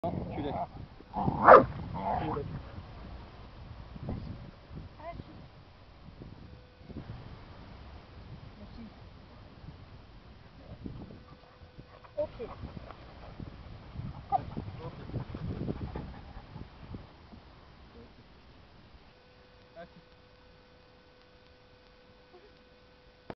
Tuto Agility (Entraîneur d'Agility), Passage du pneu.
Présentation d'un passage d'obstacle en Agility dans le cadre d'une formation d'entraîneur d'Agility avec le centre d formations GTAAF.